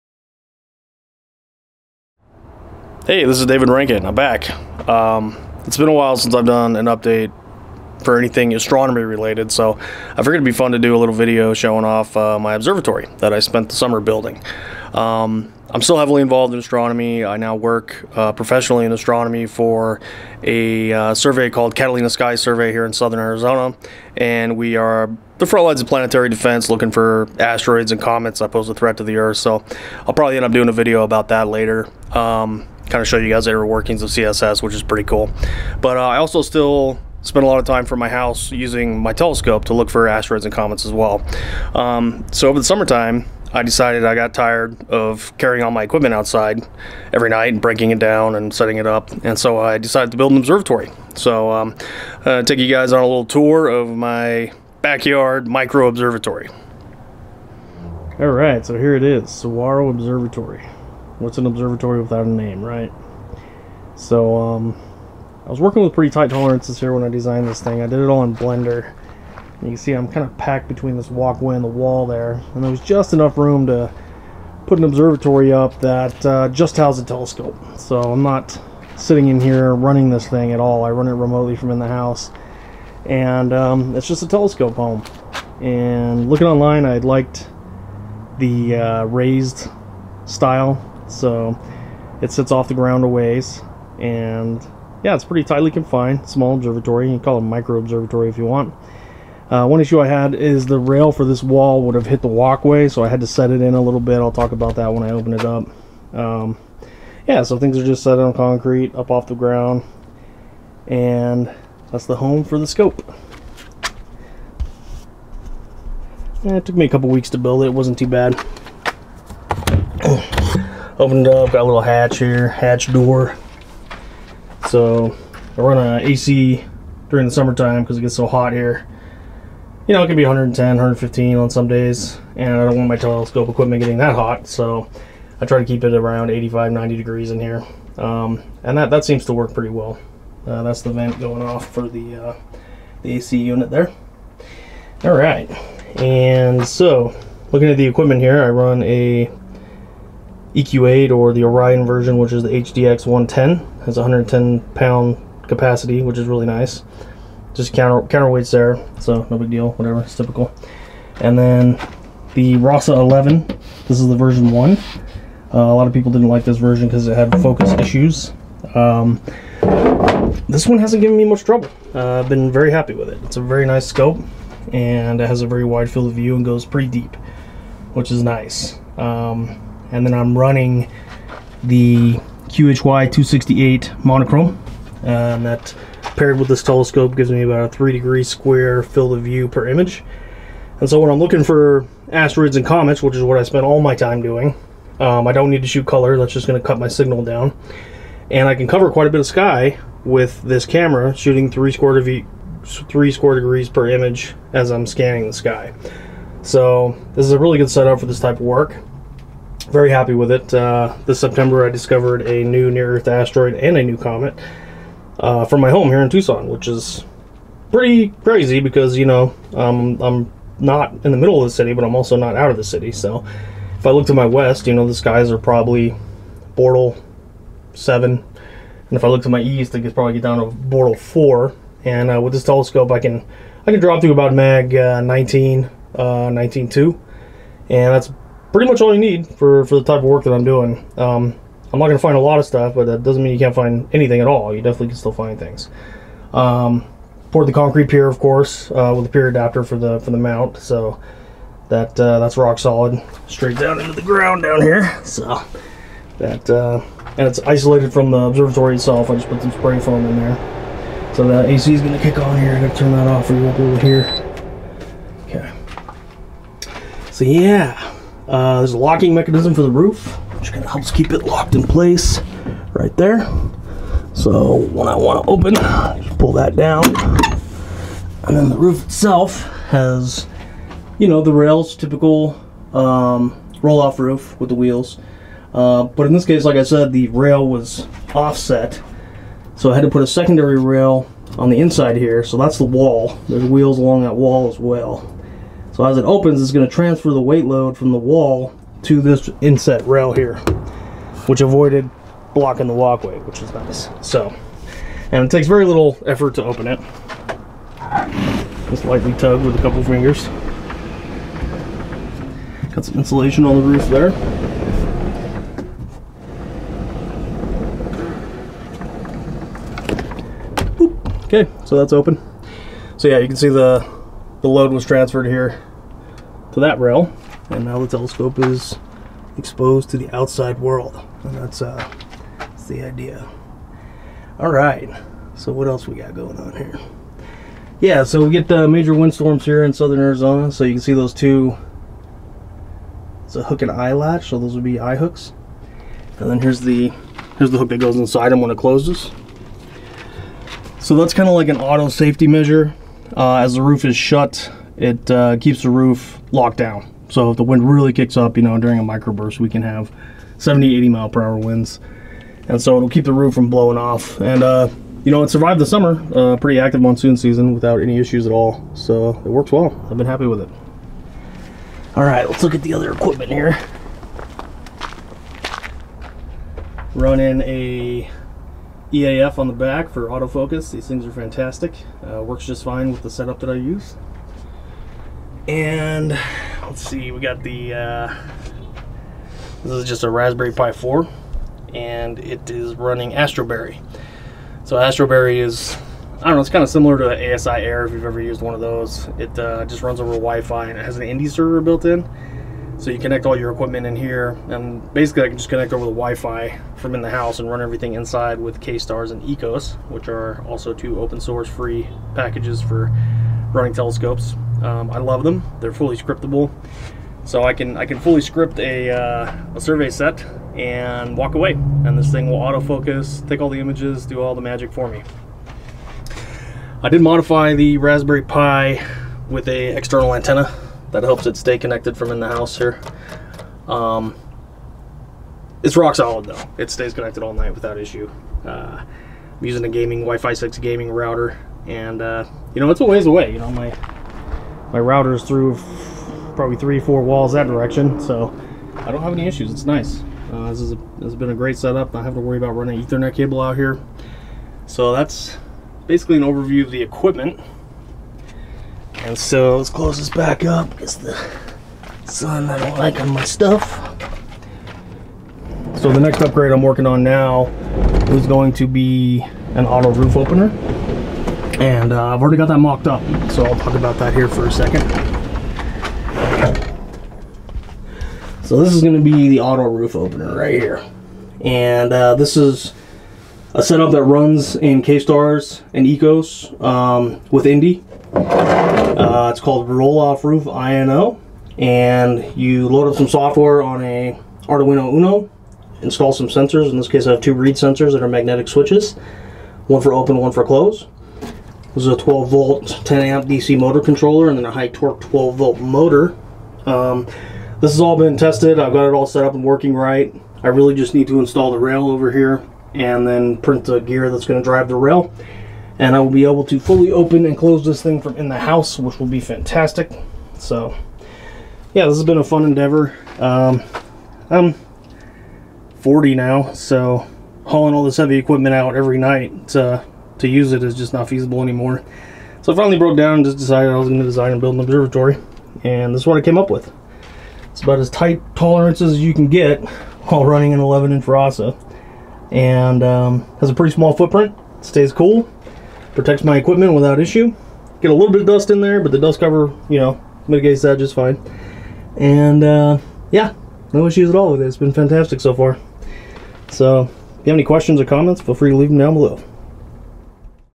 DIY Home Observatory, Roll Off Roof, Saguaro Observatory
Tour of my home observatory. Designed and built myself. Small observatory running remotely over wifi from inside the house via INDI Kstars and Ekos.

Equipment List:
Scope: RASA11 V1
Mount: HDX110 / EQ8
Focuser: ASI
Camera: QHY268M
MPC Site: V11